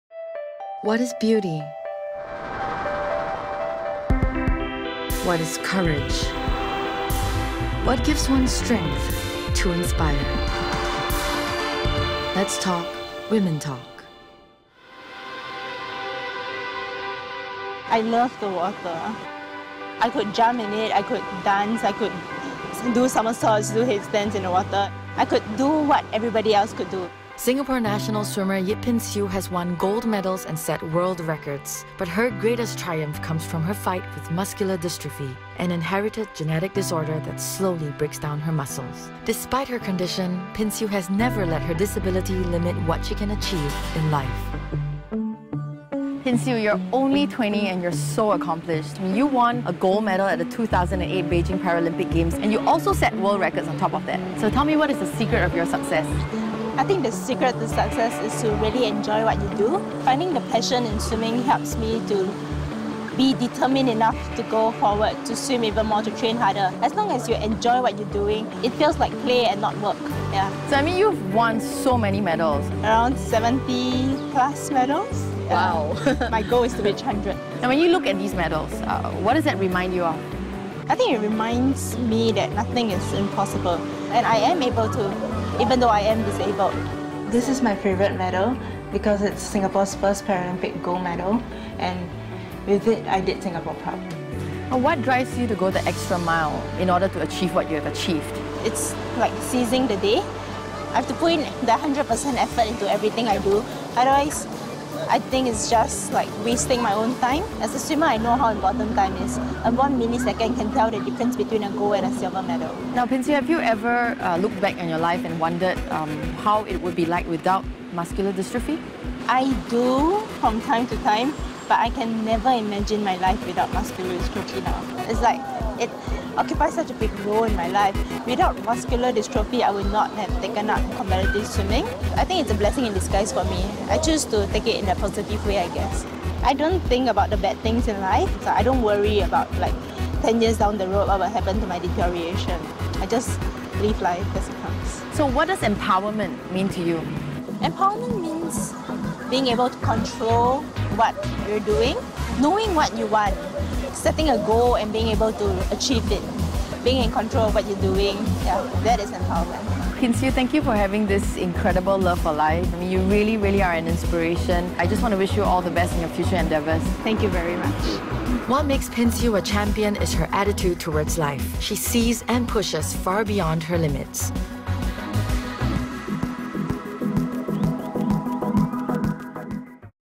Singapore Paralympic gold medalist swimmer Yip Pin Xiu's amazing story
In episode 1 of womentalk tv -
Yip Pin Xiu was born with Muscular Dystrophy, a genetic disorder that slowly breaks down the muscles. When she was five, she started swimming to improve her health and strengthen her muscles. Nevertheless, by the age of eleven, she had lost her ability to walk and had to use a wheelchair.

Despite this, Pin Xiu won Singapore's first ever Olympic gold medal at the Beijing 2008 Paralympics in the 50-metre backstroke finals. Her time of 58.75s was way ahead of Britain's Fran Williamson who took the silver in 1:06.07s. What drives Pin Xiu? What keeps this inspiring Singaporean going? And has she ever wondered what her life would be like without Muscular Dystrophy?